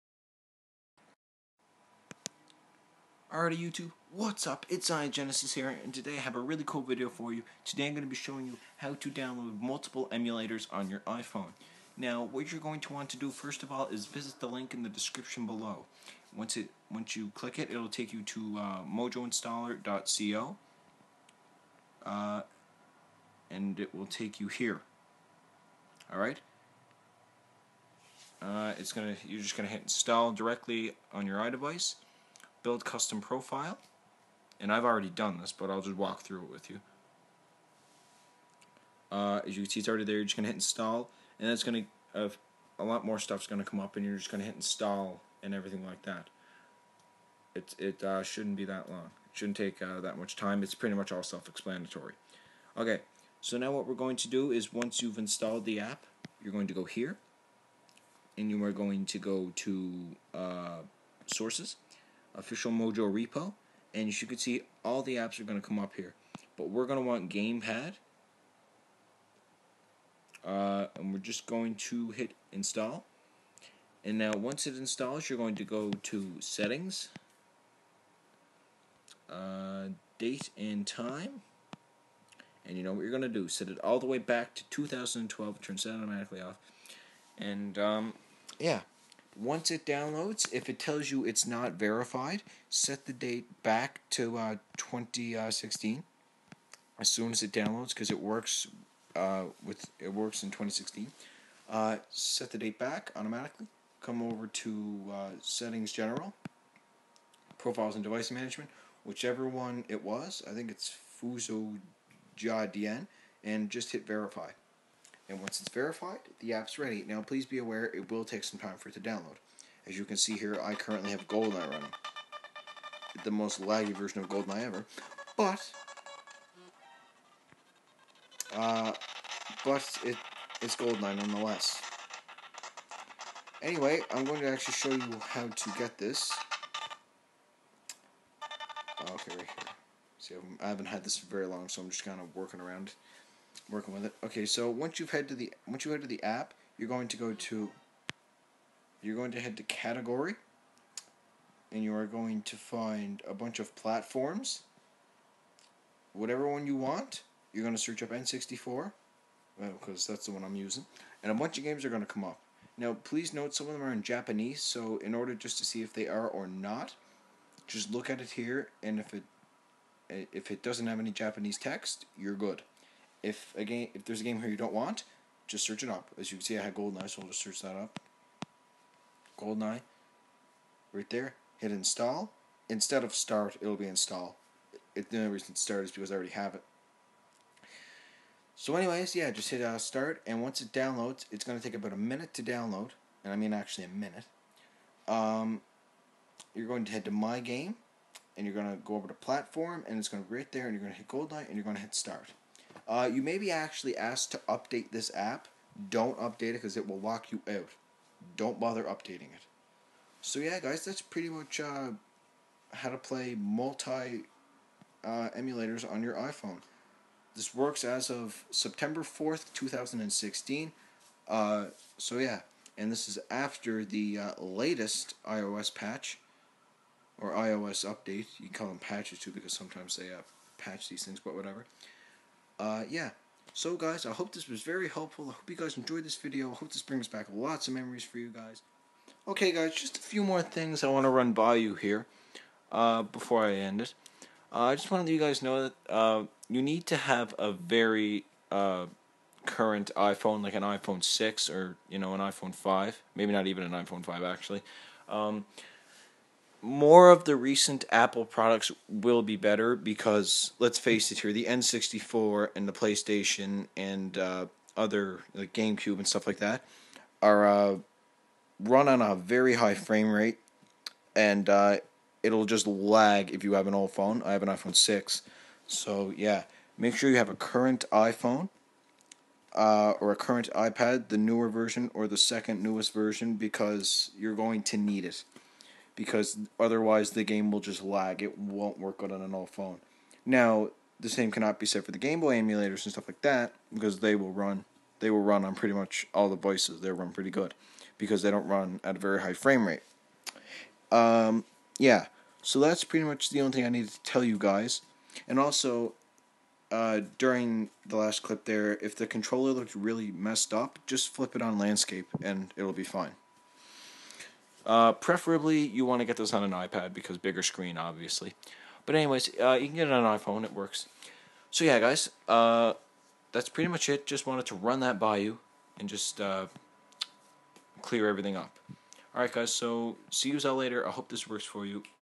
How to install multiple emulators on any iOS devices as of iOS 9.3.4
Download link

I hope this works and I hope you enjoy the video guys any problems or any questions leave them in the comments and I'll answer